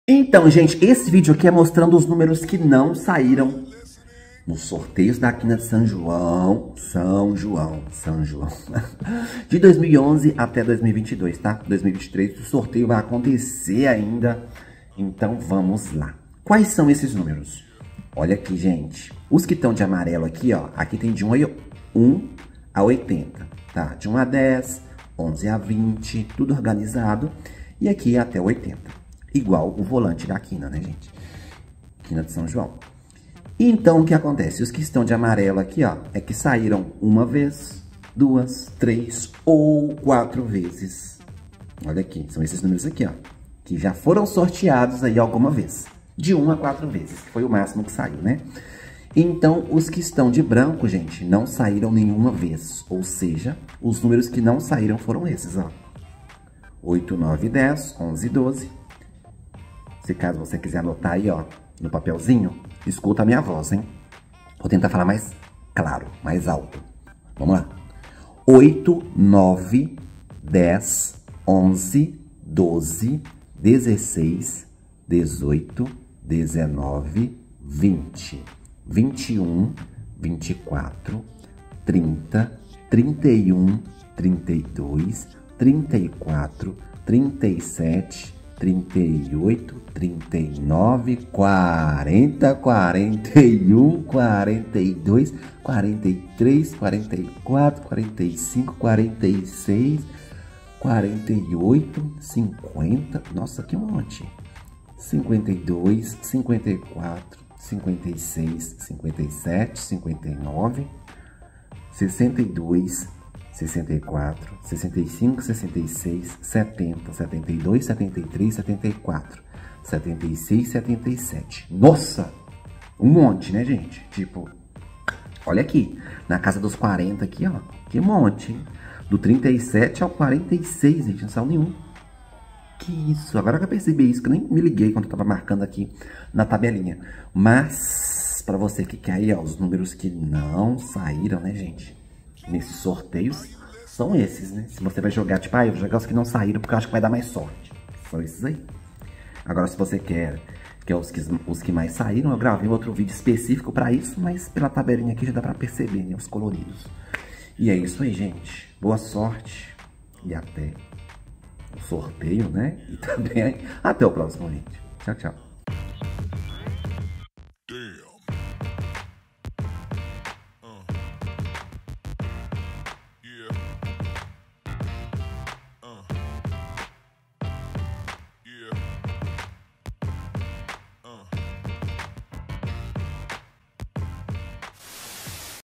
😱 TEVE NÚMEROS QUE NUNCA FORAM SORTEADOS DESDE O PRIMEIRO CONCURSO DA QUINA DE SÃO JOÃO 🍀 VEJA QUAIS
🟢 No vídeo de hoje, você encontra as seguintes informações:
Quais números nunca foram sorteados no concurso especial quina de são joão, fiz uma planilha com algumas informações.